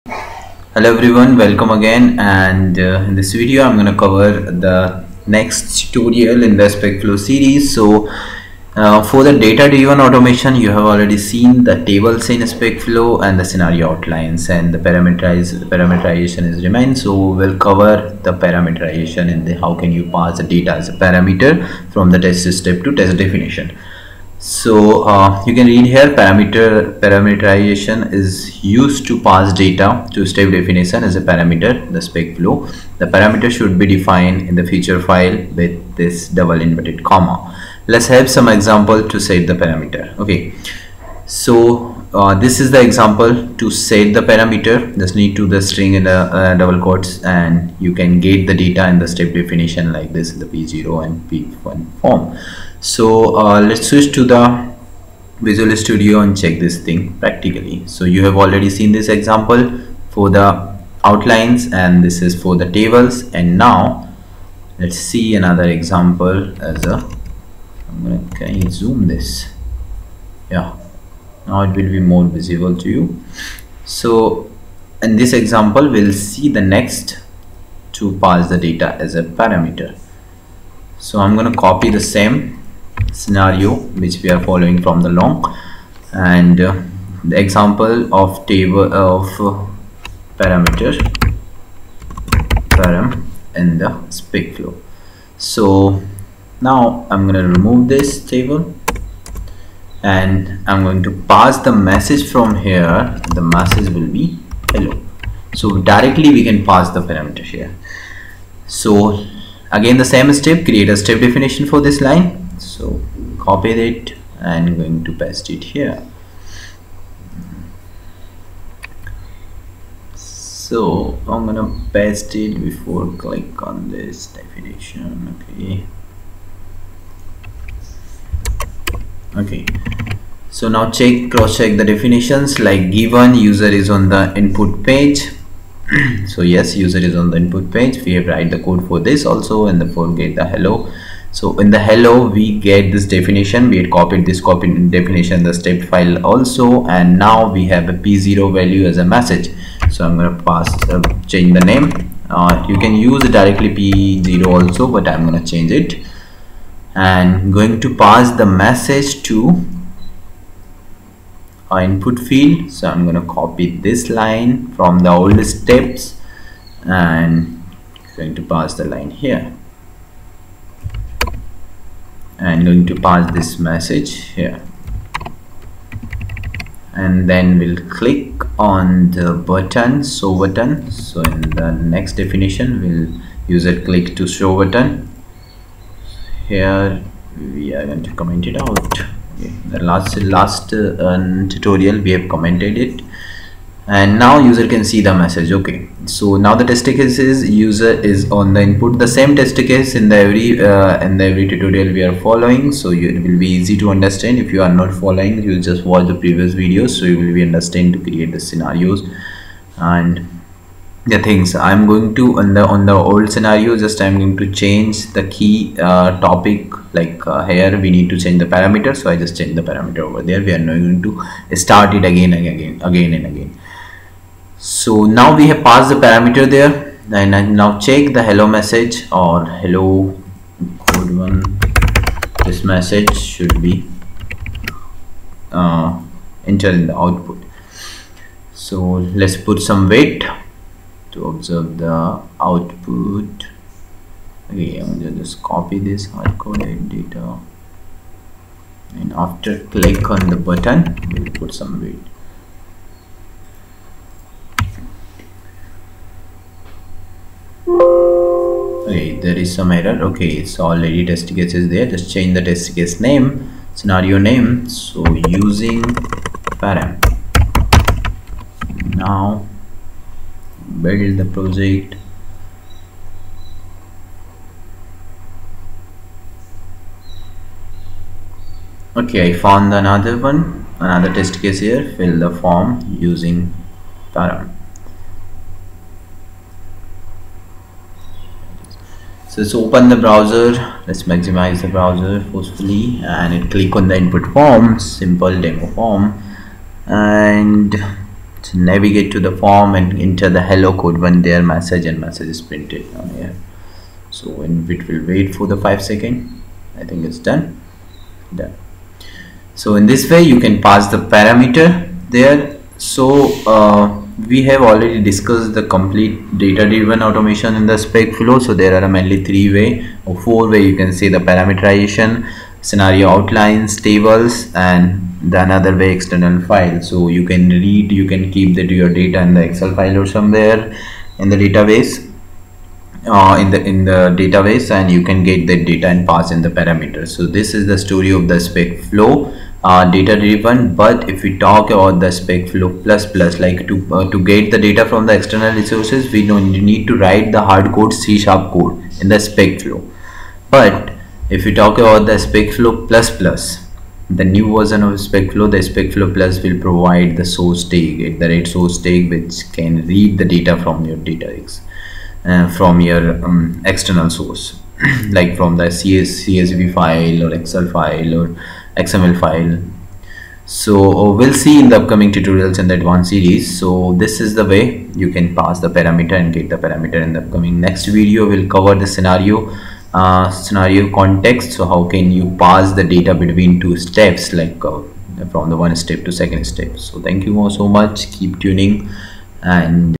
Data Driven Automation using Parameterization in Specflow
Data drive automation using parameterization in Specflow. Data driven automation in BDD with example.